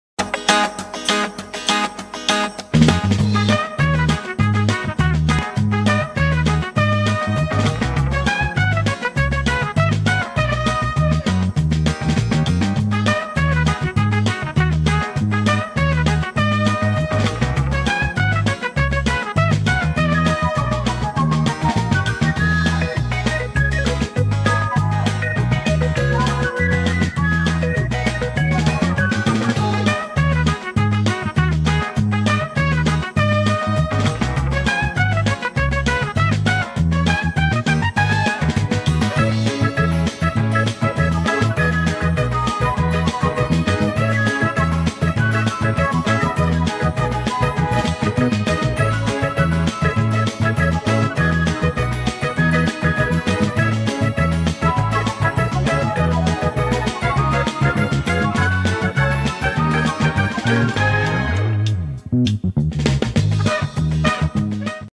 إنه يدلك جسم زبائنه بالنار !🔥😳😨🔥
هذا المُدلك المصري يستخدم النار لتخفيف آلام العضلات !🔥😳😨🔥